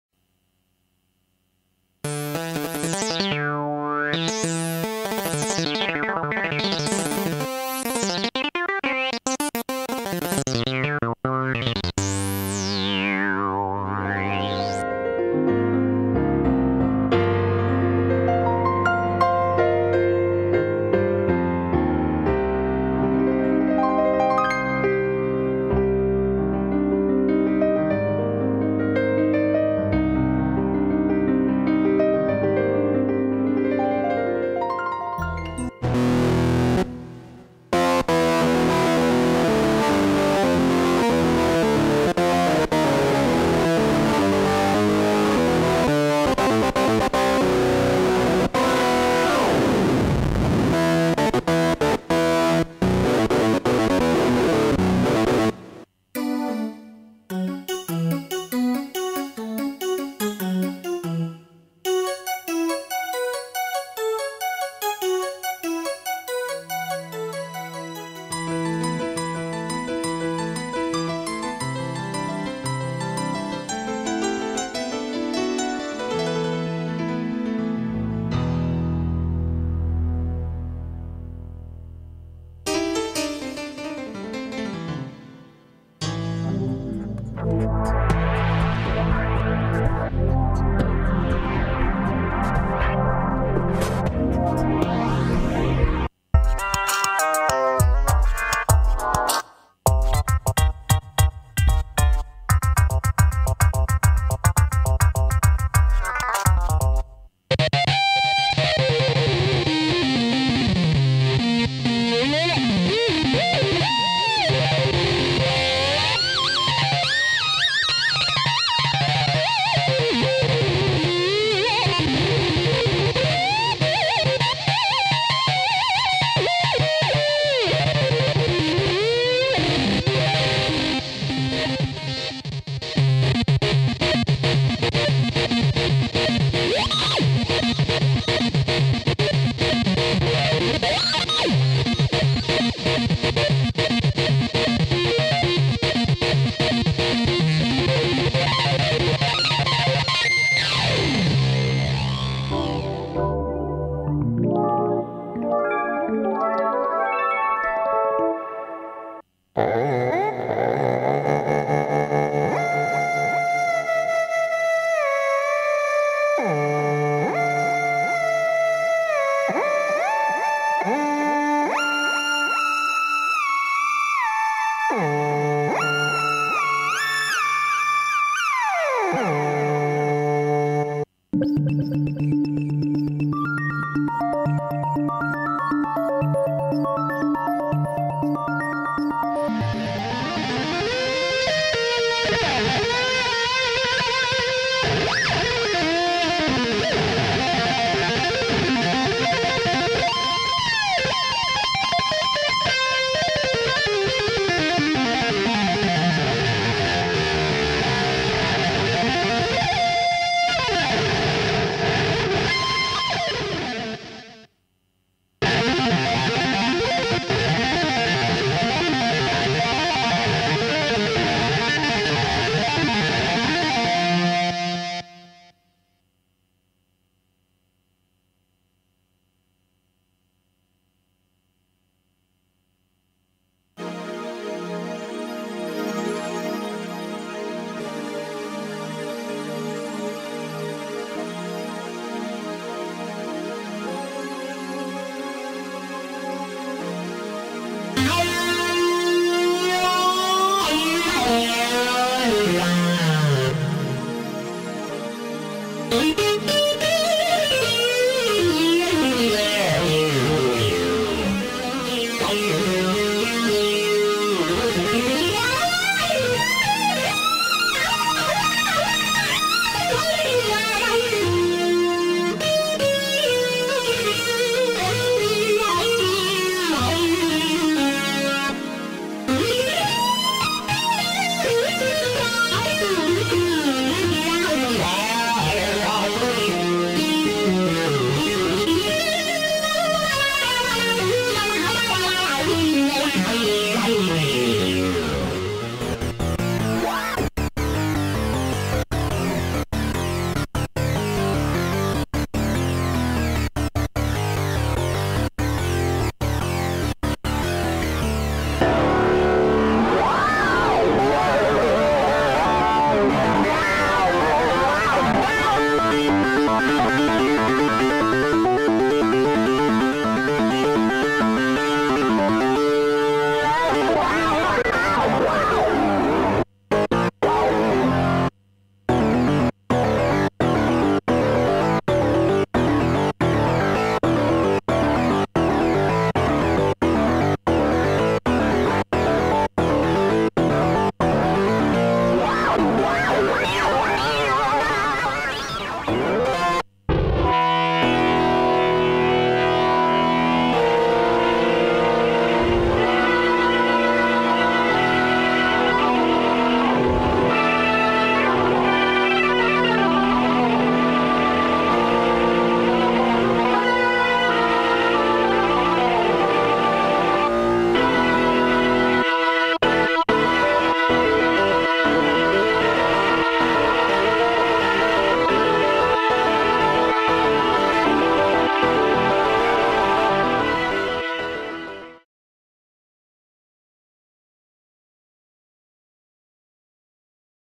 KEYBOARDS RIG | K2600, MOTIF XS, JD990, V-SYNTH, TRINITY, NORD LEAD 3
▬▬▬ Download Link ▬▬▬▬

Features:
Kurzweil K2600
Yamaha Motif Xs
Roland Jd990
Roland VSynth
Korg Trinity Custom+Triton Module
Clavia Nord lead 3